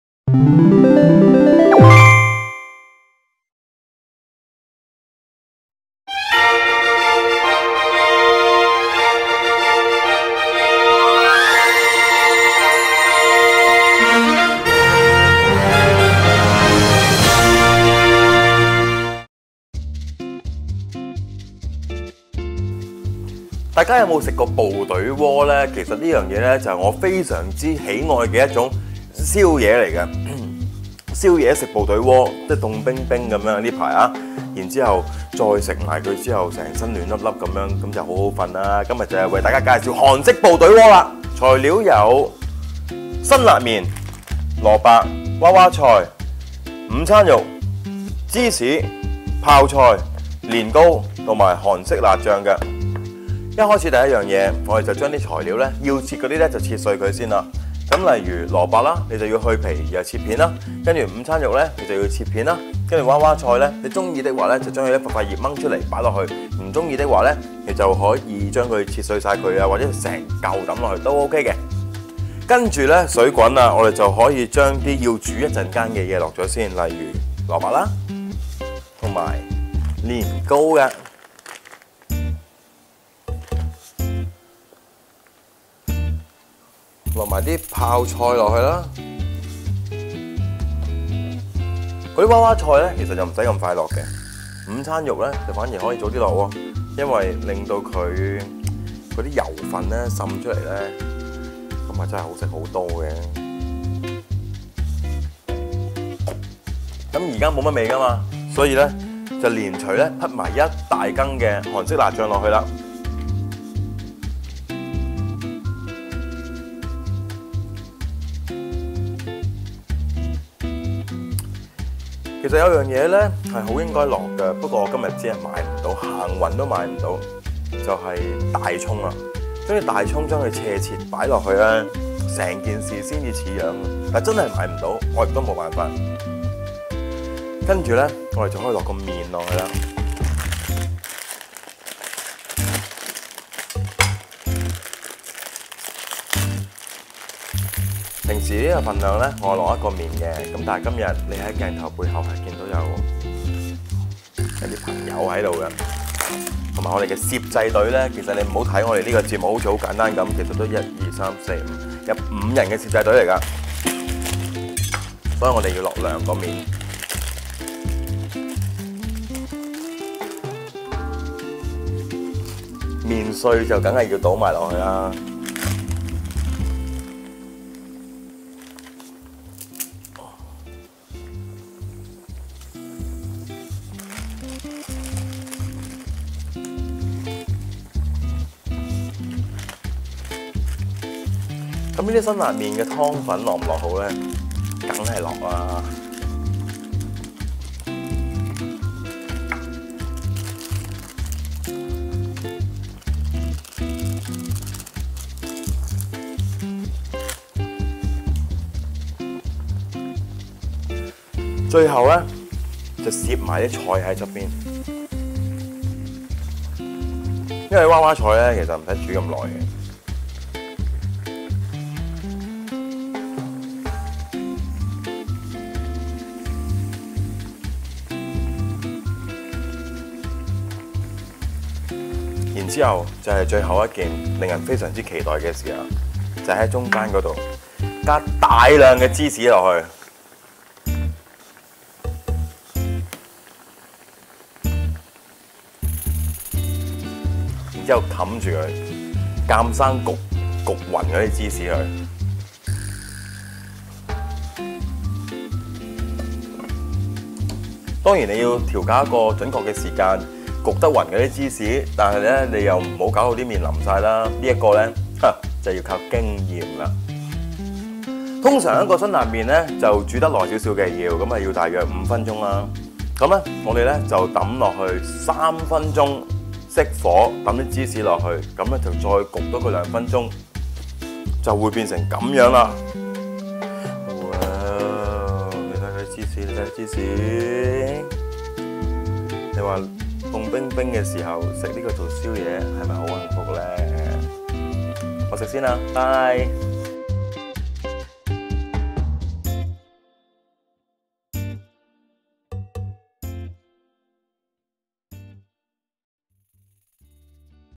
梁祖堯 Joey's Kitchen - (U Magazine Issue 536)
梁祖堯 Pumpkinjojo：「睇韓劇成日都會見到啲演員喺戲裏面食部隊鍋，食到津津有味，深宵煲劇愈睇愈肚餓！」